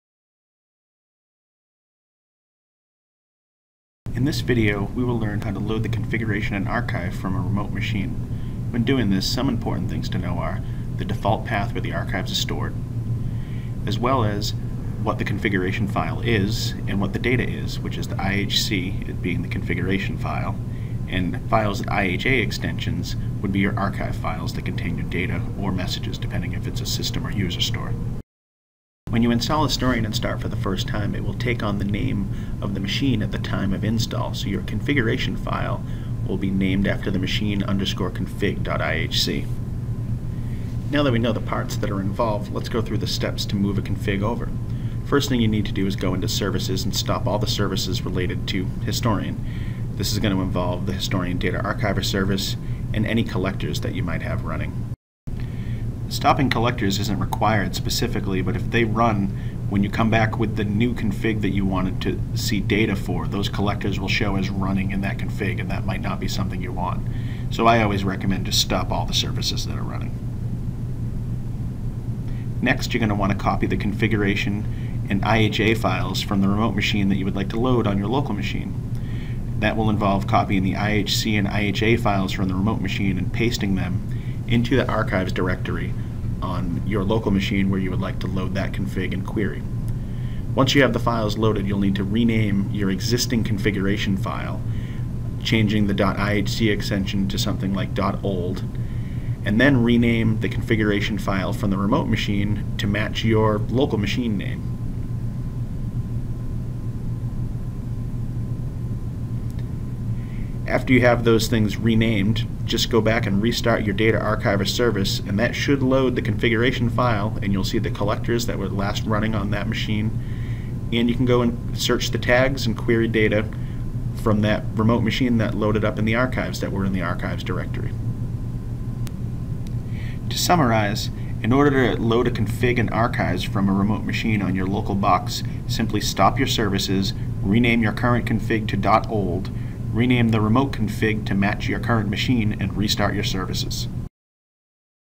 GE Training: How to Load Historian Configuration and Archive Files
This video focuses on GE Proficy Historian.  In just 2 minutes, you can learn how easy it is to load the configuration and archive data from a remote Historian server. This can be helpful when troubleshooting where you might want to review the configuration or can be a useful way to query data without adding stress to a production machine querying data and/or messages remote from the production environment.